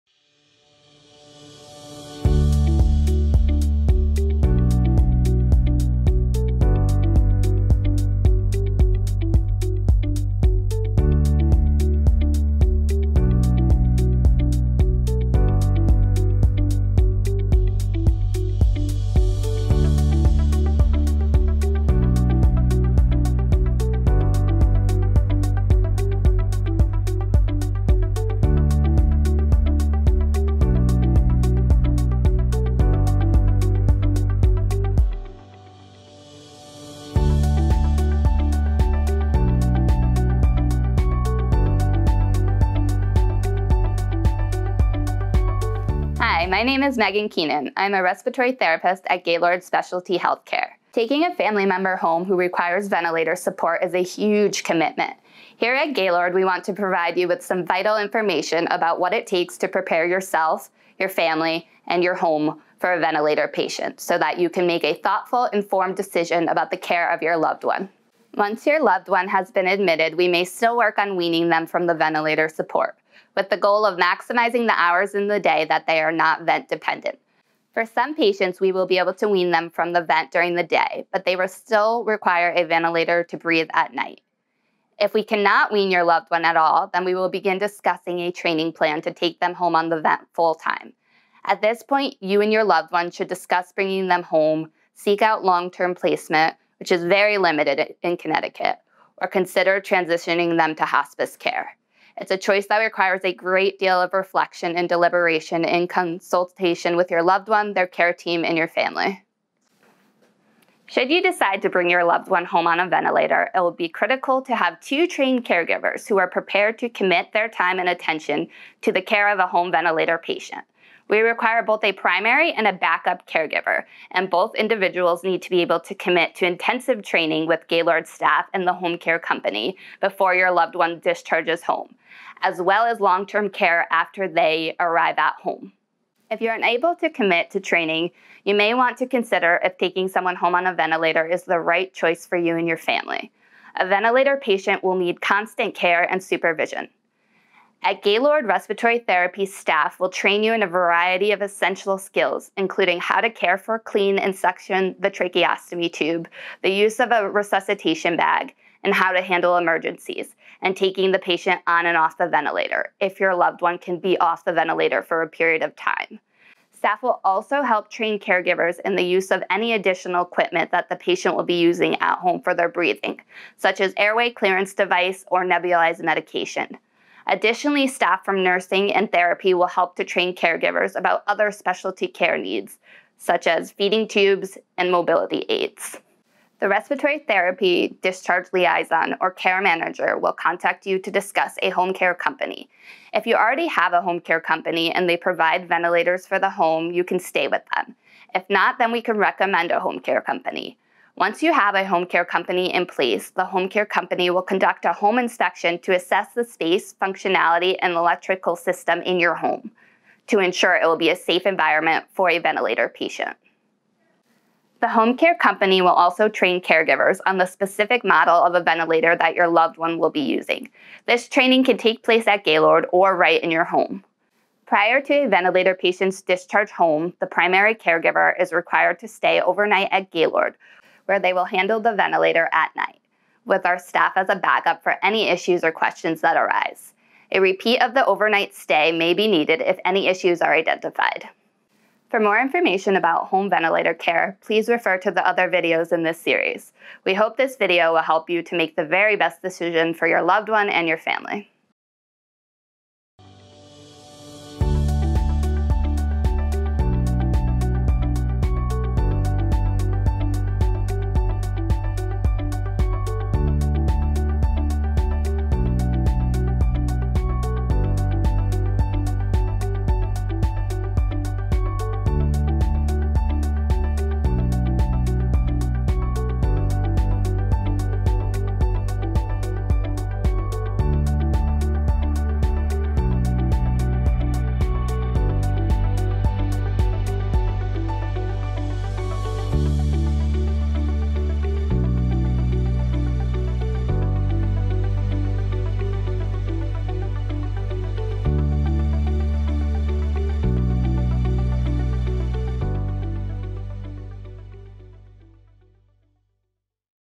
Vent Program Video 2: Training Caregivers
Gaylord's Destination Vent/ Home Ventilator Training Program will train family members in providing home ventilator care for a loved one who is unable to be weaned or can only be partially weaned from the vent. Caring for a loved who is vent dependent is a 24-hour per day, 7 day a week commitment. Home care aides or nurses will not care for the vent. At Gaylord, we will train family members about how to care for a vent dependent loved one in the home setting. Info on our Vent Weaning Program